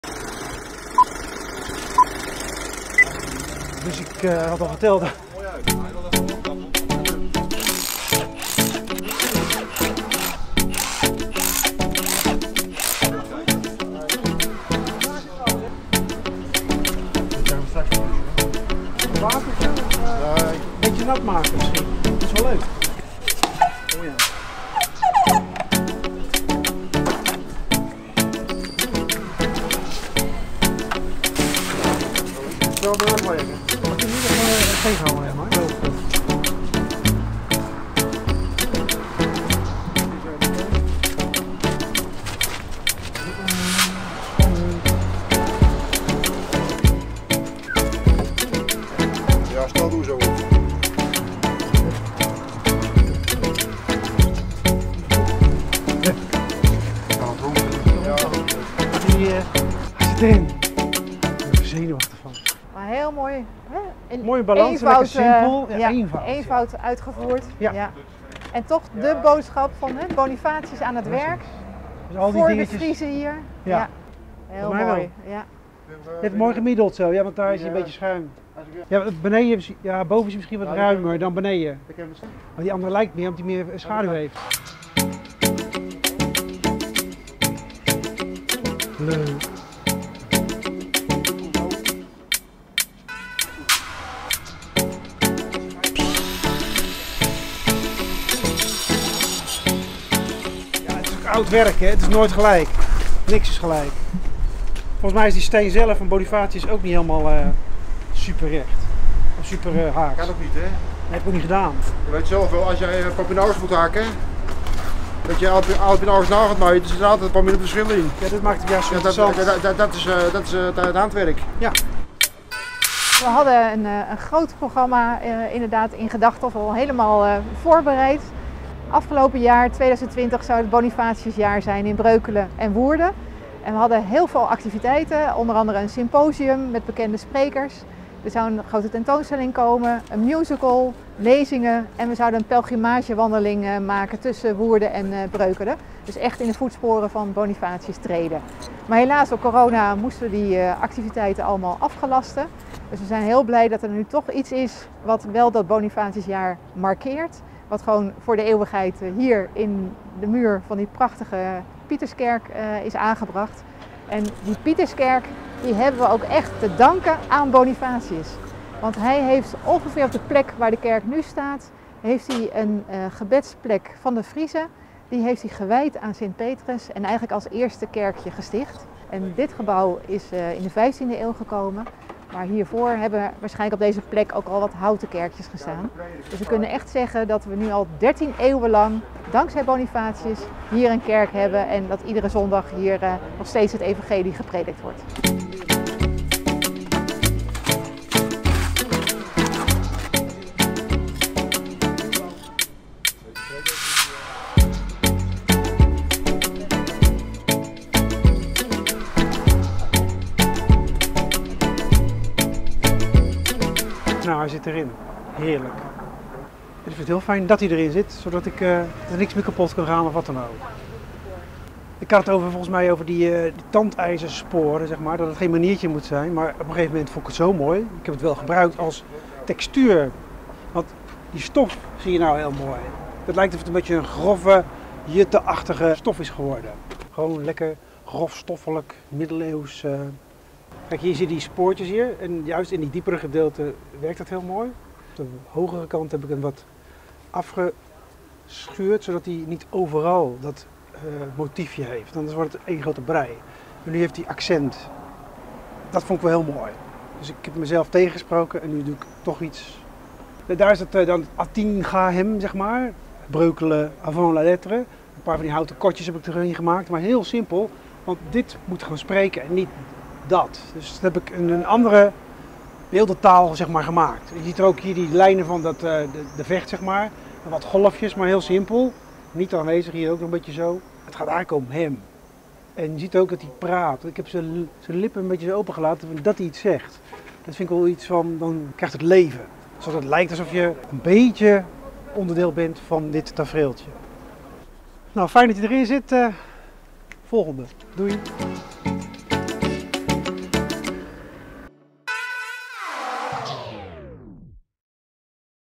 SERGE VAN DRUTEN - BEELDHOUWER VLOG 4
De Bonifatius gevelsteen is klaar en wordt in  muur gemetseld van de St. Pieterkerk in Breukelen